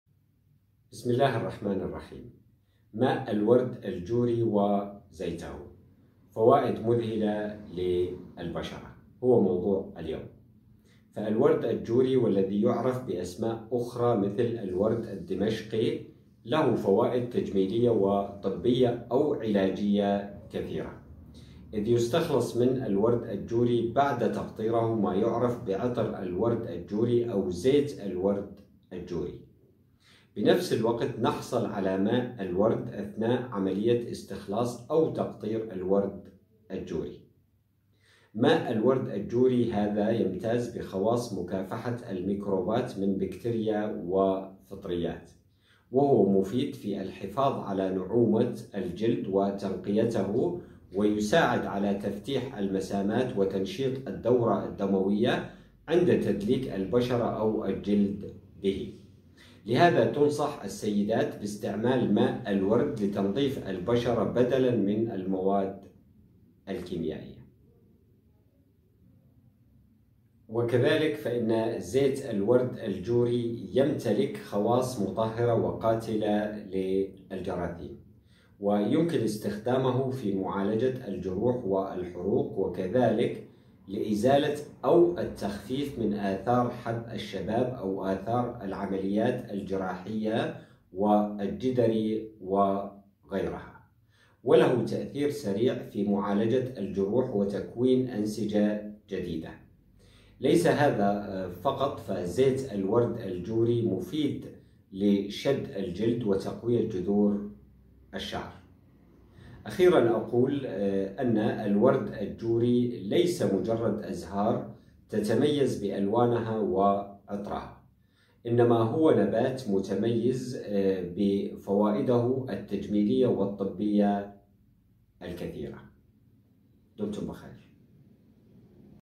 ماء الورد و زيت الورد الجوري فوائد مذهلة للبشرة ......
ماء الورد الجوري و زيت الورد الجوري فوائد مذهلة للبشرة.
الورد الجوري أو مابعرف بالورد الدمشقي له فوائد طبية وعلاجية كثيرة إذ يستخلص من أزهار الورد الجوري مابعرف بعطر الورد الجوري أو زيت الورد الجوري وبنفس الوقت يستخلص ماء الورد أثناء عملية تقطير أزهار الورد الجوري.
ماء الورد الجوري يكافح البكتريا والفطريات ومفيد في الحفاظ على نعومة البشرة وتنقيتها ويساعد على تفتيح المسامات وتنشيط الدورة الدموية لذلك تنصح السيدات باستخدام ماء الورد لتنظيف البشرة.
أما زيت الورد الجوري فيمتلك خواص مطهرة ويمكن استخدامه في معالجة الجروح والحروق وله تأثير سريع في معالجة الجروح وكذلك يستخدم للتخفيف من آثار حب الشباب وآثار العمليات الجراحية ويستخدم أيضا لشد الجلد وتقوية جذور الشعر.
الدكتور أحمد الشلاش العبيد